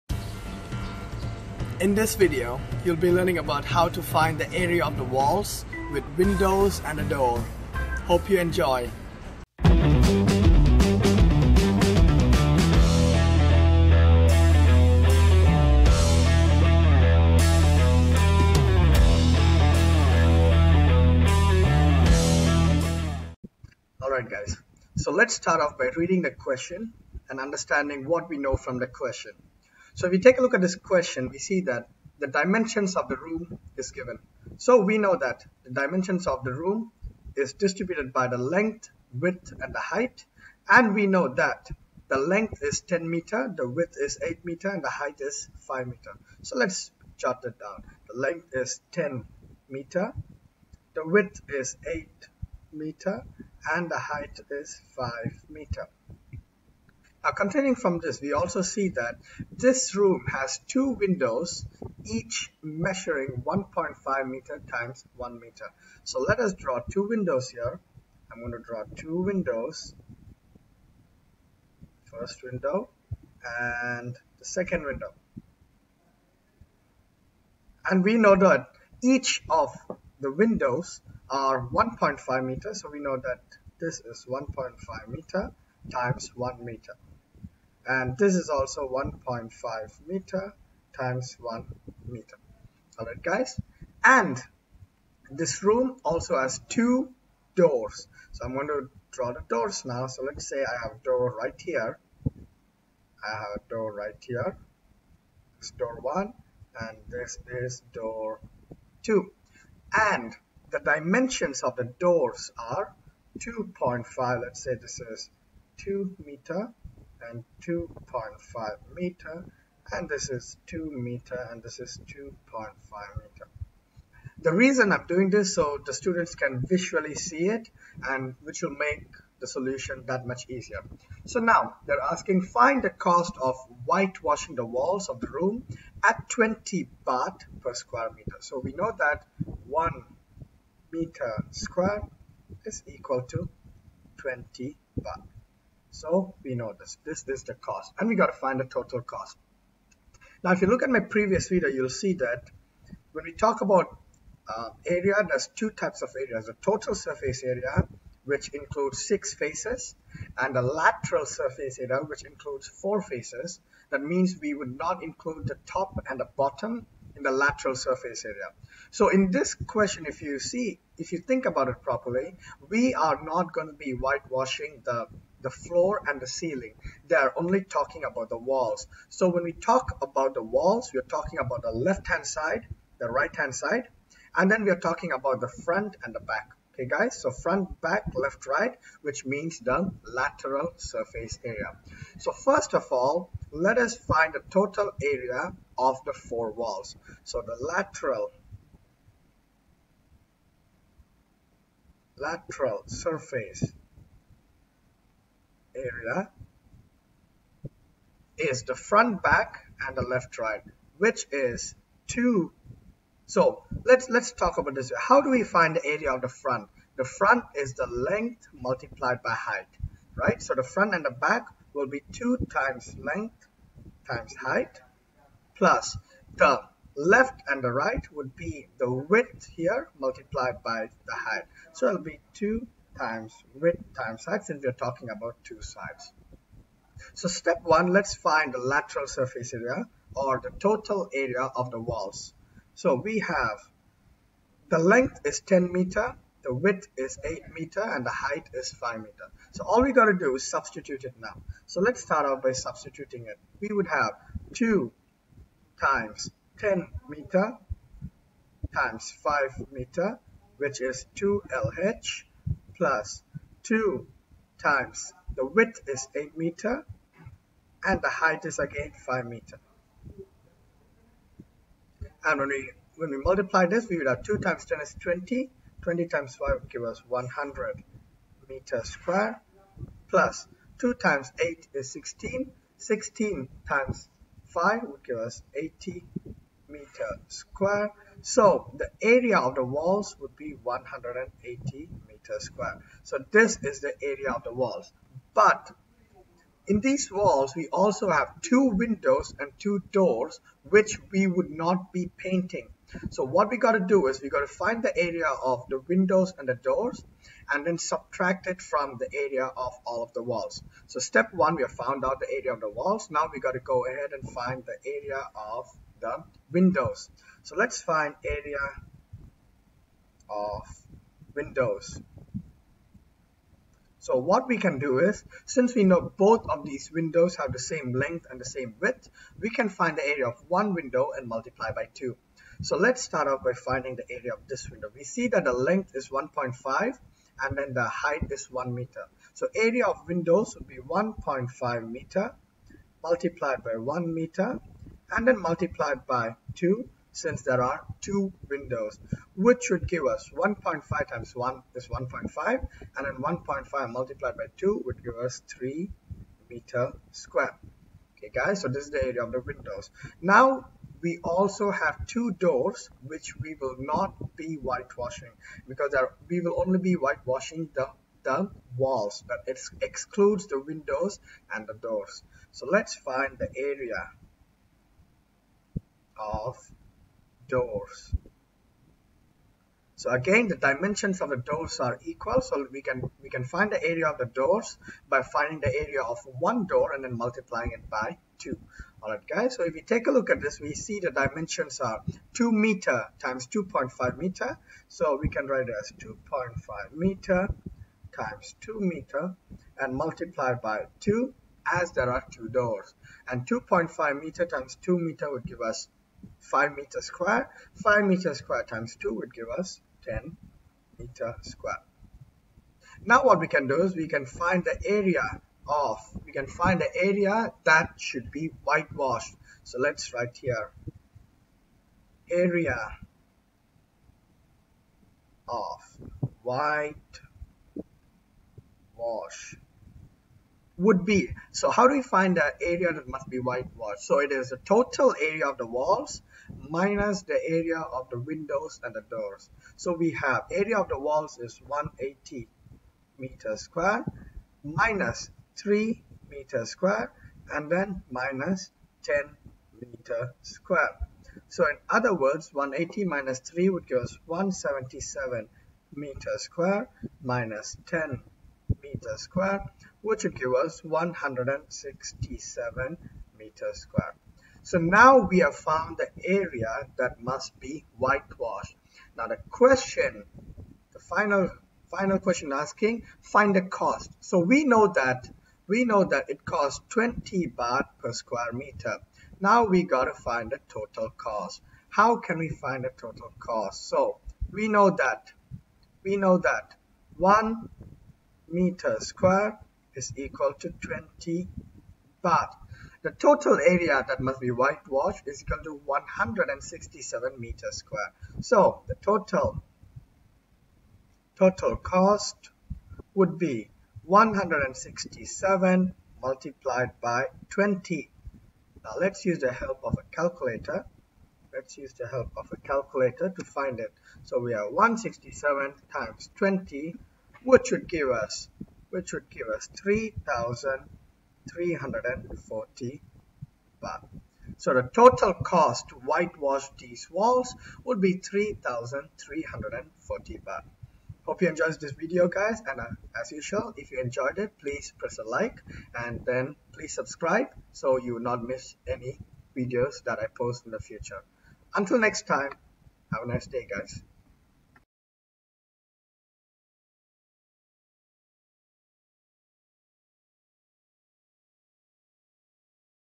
Calculate Surface Area of Rectangular Prism (With Doors and Windows)
In this Math video, I will be explaining how to calculate the area of a rectangular prism that has windows and a door. I will start off by finding the Area of the walls followed by finding the area of the windows and the doors. After this step, I will subtract the area of the windows and the doors with the area of the walls

Question) The dimensions of a room are 10m x 8m x 5m. This room has two windows each measuring 1.5m x 1m and two doors measuring 2.5m x 2m. Find the cost of whitewashing the walls of the room at 20 Baht per sq.m.

The method I use is not to let the students memorize the formula but to understand how to find it. The total surface area of a rectangular prism can be found by finding the surface area of FRONT/BACK, LEFT/RIGHT & TOP/BOTTOM and adding it. The Lateral surface area of a cuboid can be found by finding the area of FRONT/BACK & LEFT/RIGHT.

Watch Math tutorials with Maths with Teacher.Satya